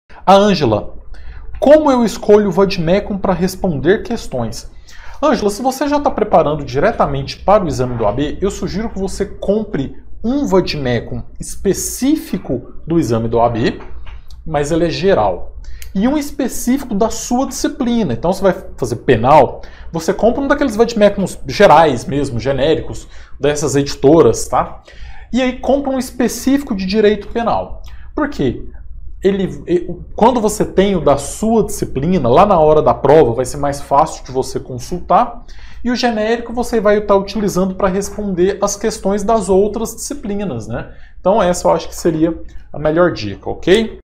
Vade ideal para responder questões.
Nesse vídeo eu te explico como escolher seu material de consulta na hora da prova!